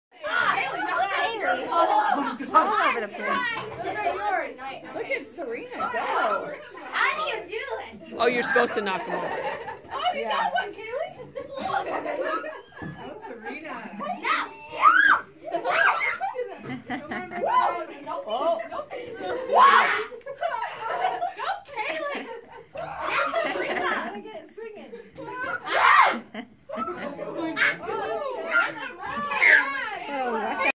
Fun games
Put nylons on your head and try to knock over water bottles with the tennis ball that you put in the other end.